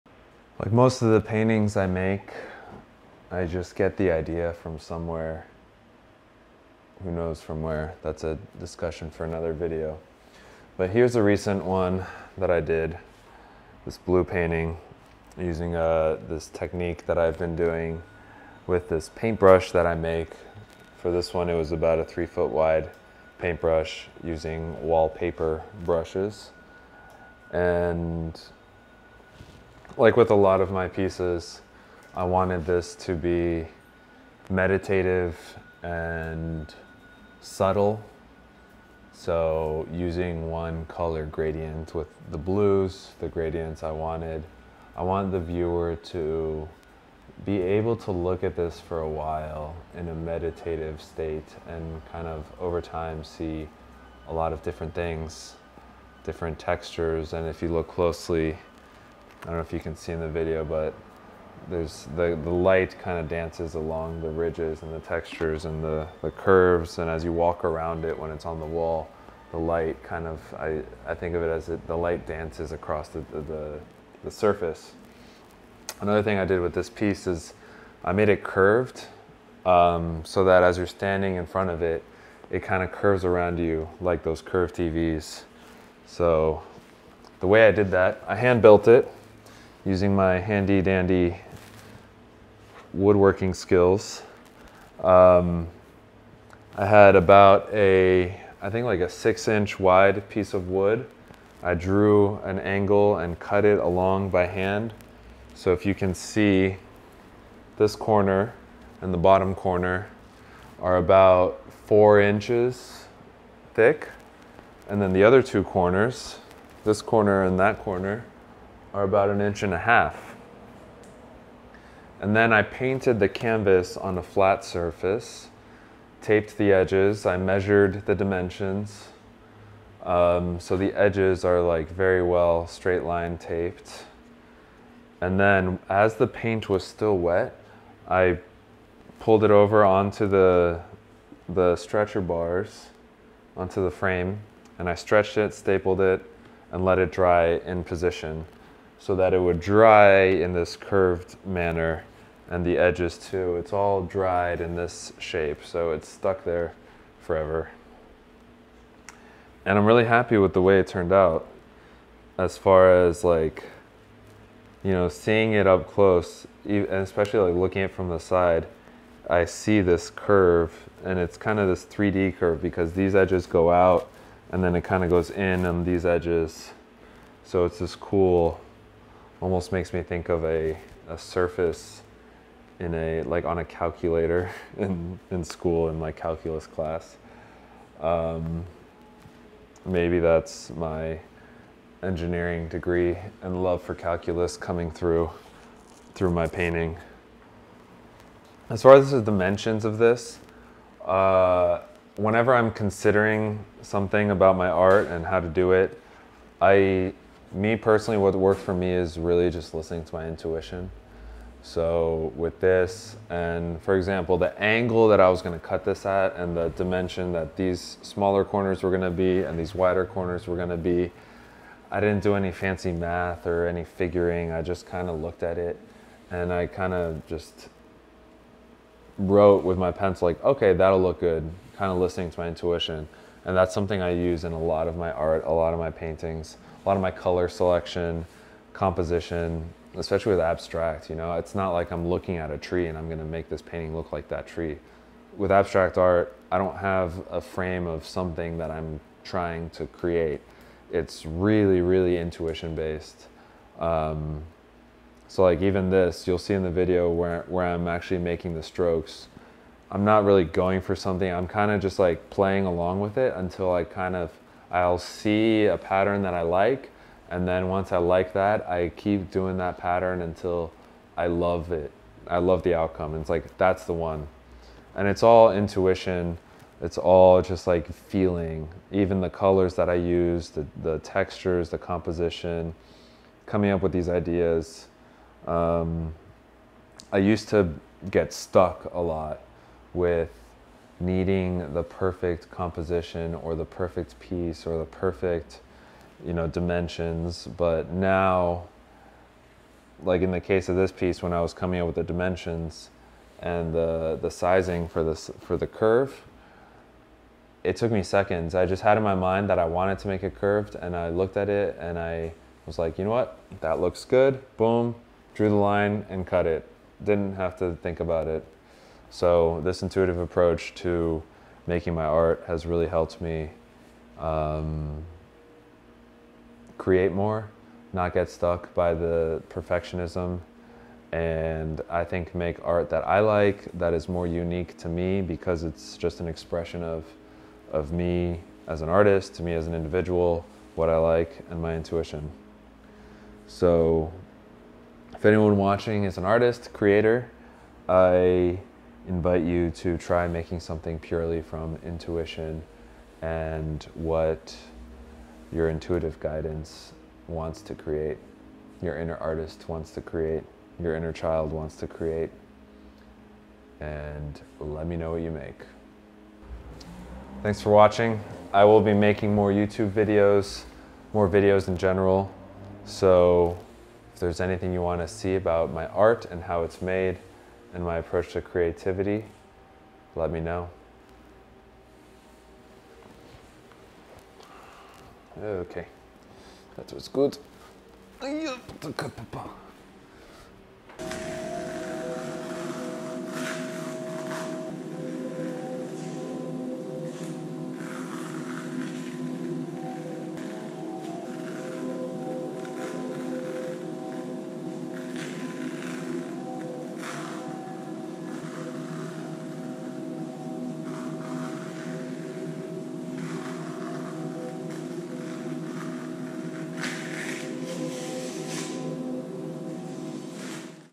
Making a Curved Oil Abstract Painting
In late 2021, I had the idea to make some curved oil paintings. Here is a video describing my process in building one curved frame by hand, how I painted it, mounted the canvas, and how I listen to my intuition as creative guidance.

0:00 - Intro
0:15 - Technique Overview
1:15 - Making the Curved Canvas
3:17 - Using Intuition to Create Art
6:06 - Outro
6:30 - Tape Peeling ASMR